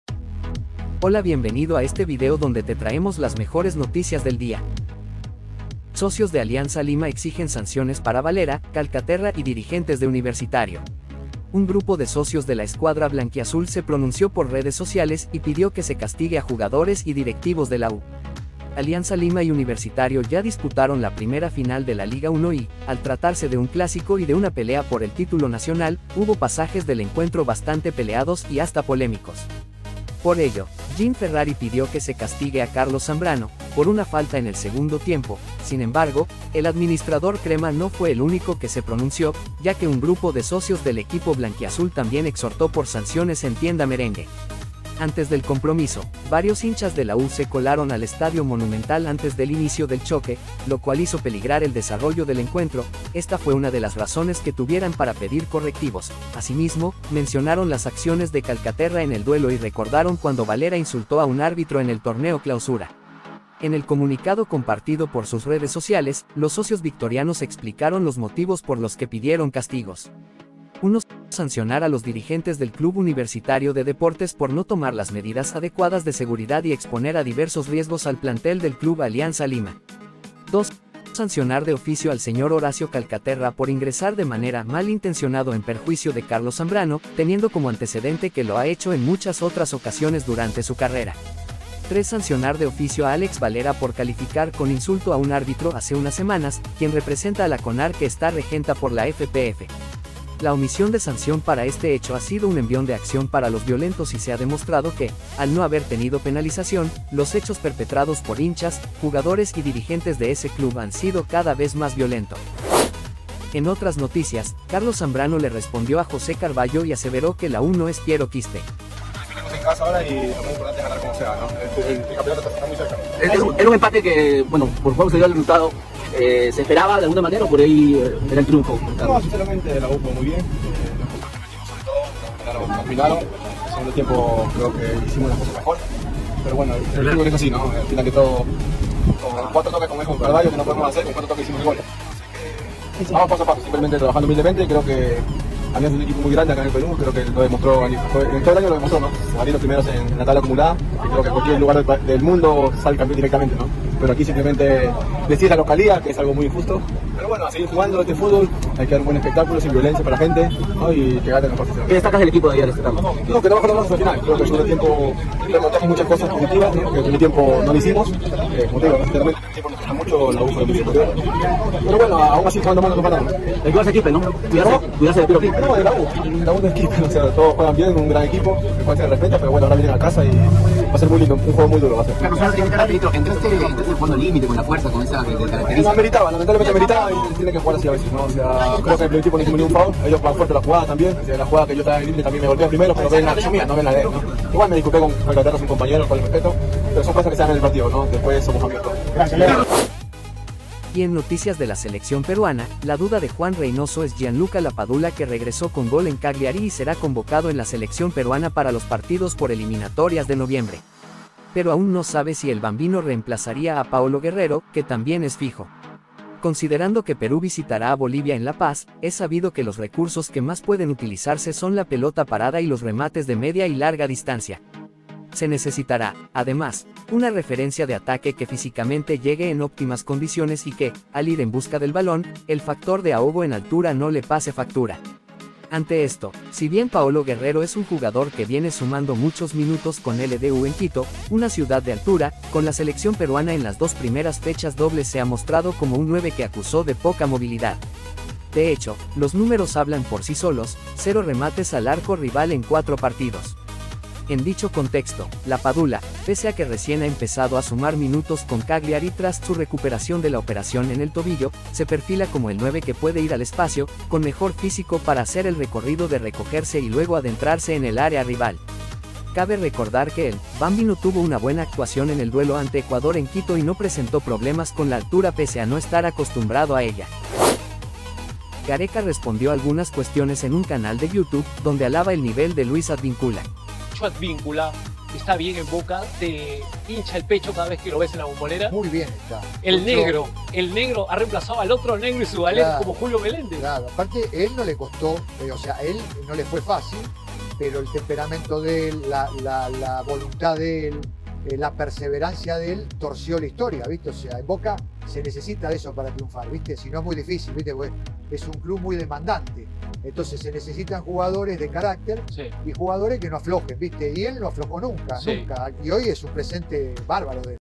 Alianza Lima DEMANDA A UNIVERSITARIO 🔥 GARECA ELOGIA a Advincula | CARLOS ZAMBRANO Calienta CLÁSICO
Alianza Lima hace DEMANDAS CONTRA UNIVERSITARIO de deportes 🔥 GARECA ELOGIA a Luis Advincula | CARLOS ZAMBRANO Calienta CLÁSICO contra Piero Quispe.

Eliminatorias sudamericanas, Liga 1, Jugadores peruanos en el extranjero y Mucho más.